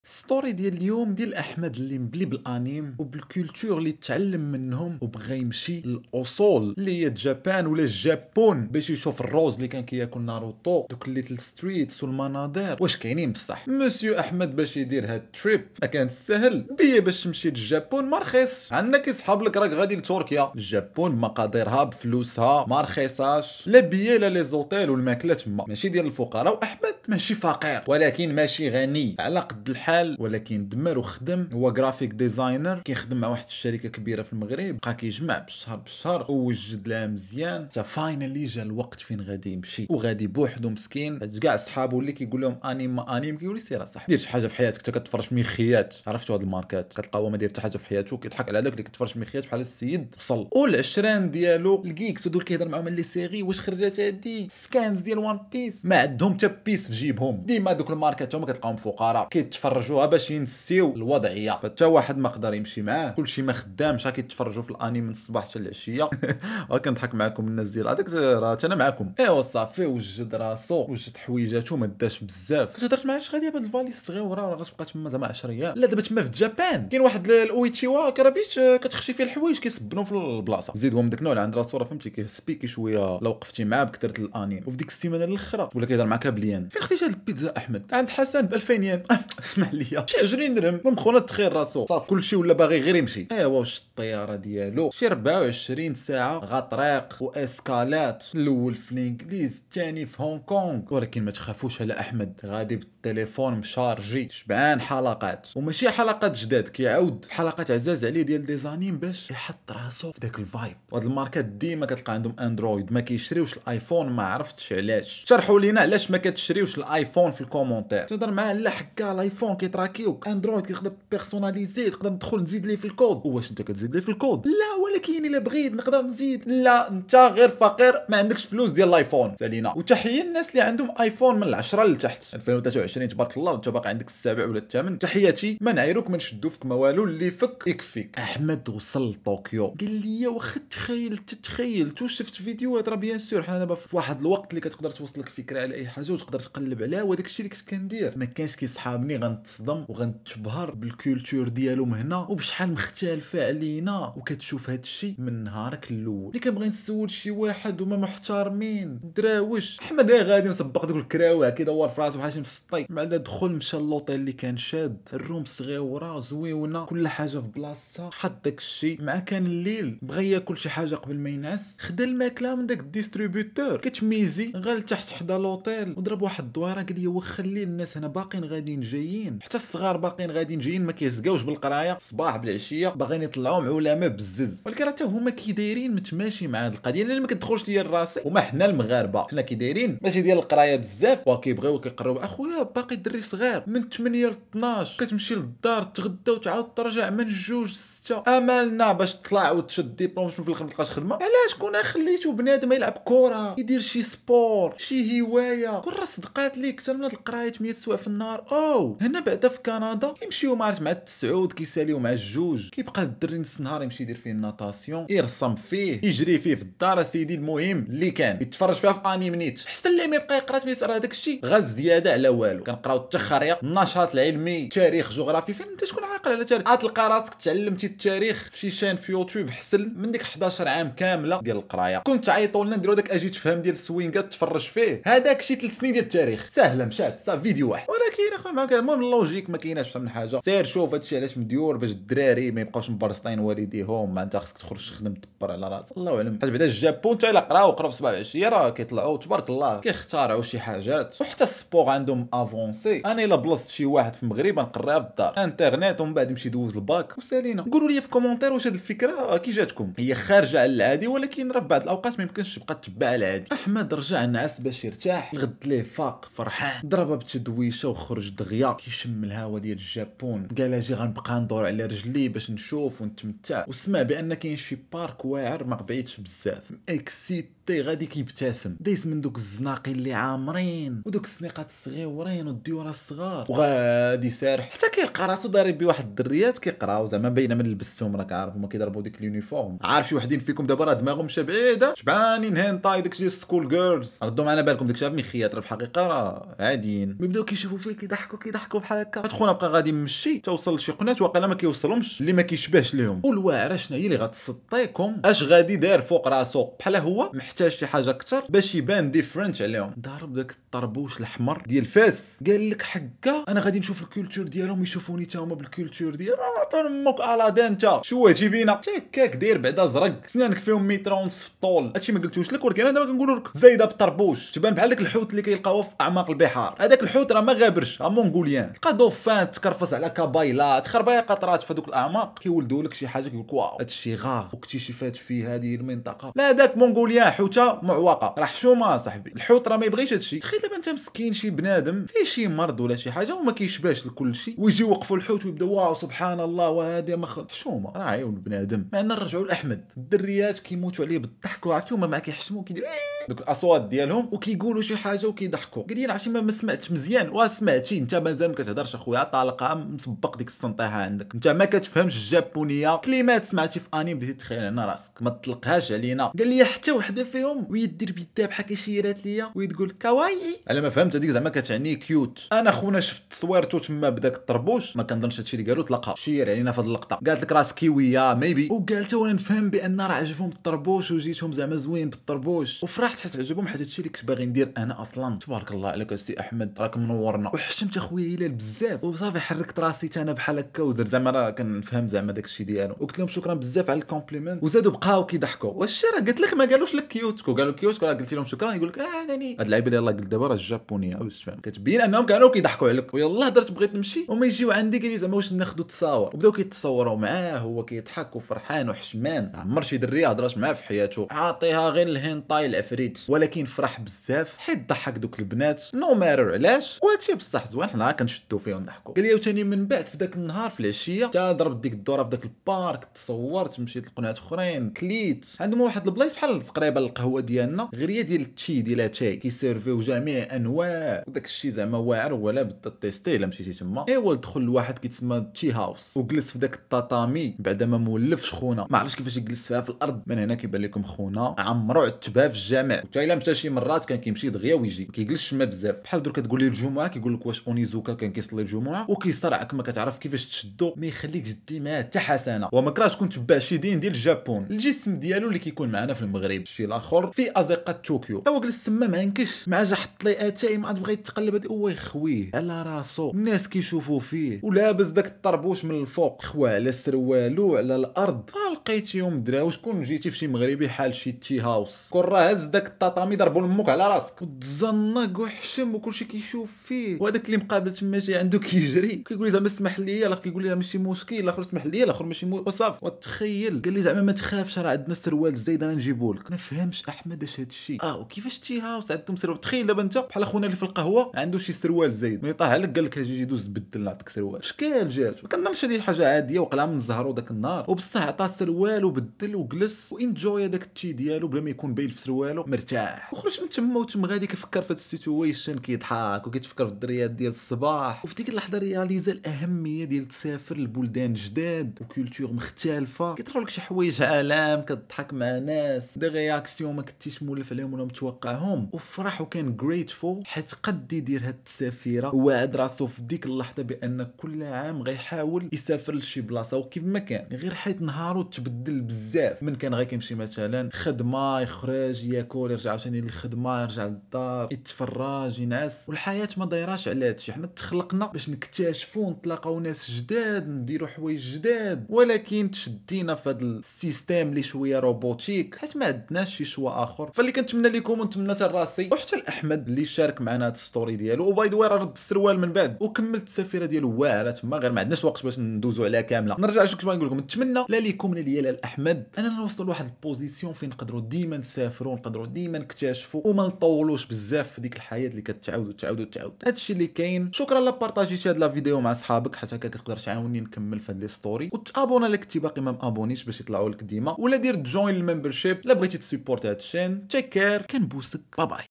بسبب الأنيم سافرت لليابان و عشت لحضات لا تنسى
Parlez Avec Hilal : Canada 🇨🇦 / Création de Contenu 🎥

cheers bogoss !